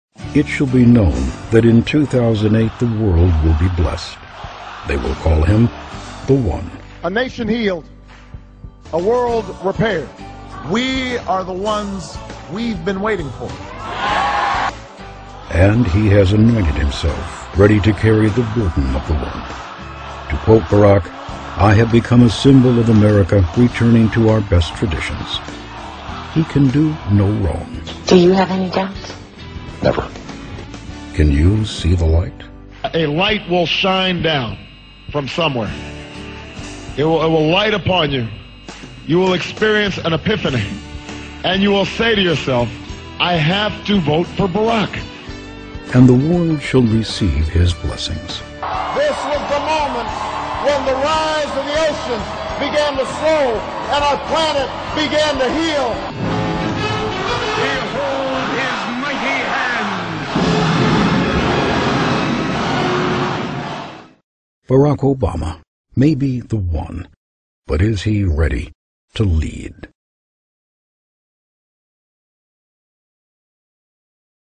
The One that 666 Marked
I bought the Chicago Tribune of the day of Obama's victory with the Lottery draw of 666 in it:

There's no denying the numbers marked Obama and that this is a heavenly supernatural mark. The chances are too great for it to be chance. And if it's chance then it's just Obama's ill-luck to be marked by this chance-destiny.